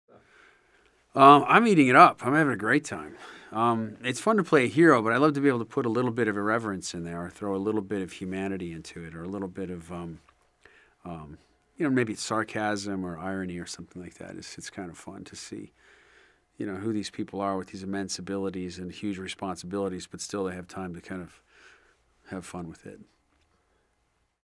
SUPERMAN: Mark Valley on Playing a Hero in Dark Knight Returns, Part 2
In this video interview, actor Mark Valley shares his feelings on playing a hero as he voices Superman in The Dark Knight Returns Part 2.